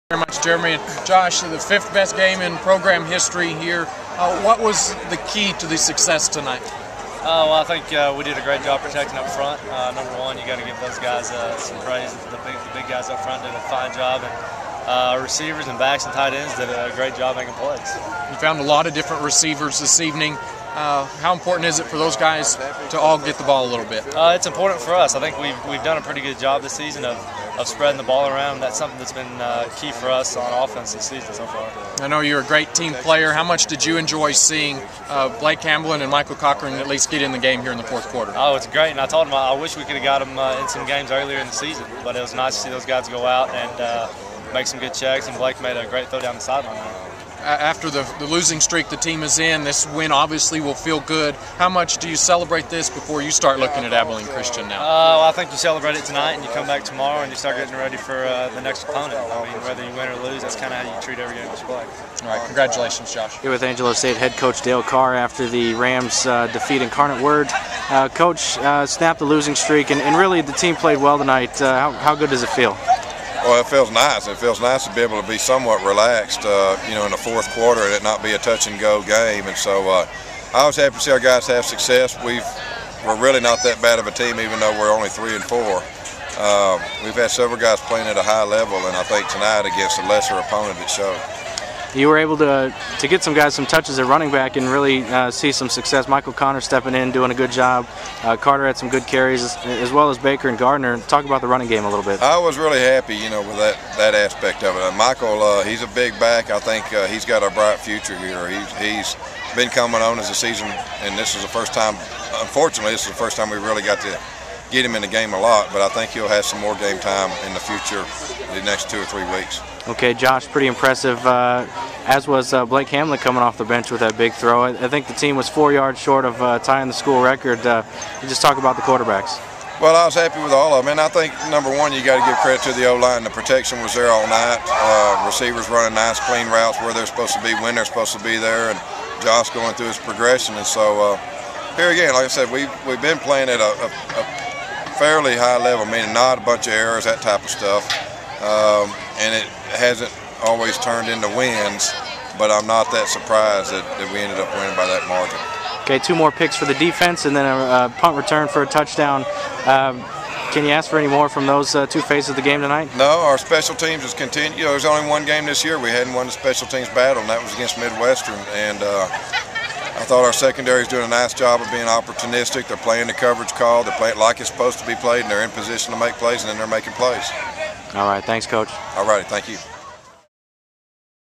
Inside AngeloSports.com: Neiswander and Carr Talk About Win over Incarnate Word (10/23/10)
Josh Neiswander threw for a season-high 385 yards and hit four different receivers for scores as Angelo State snapped a four-game losing skid and collected the second-highest point total in school history in a 61-17 win over Incarnate Word at San Angelo Stadium Saturday.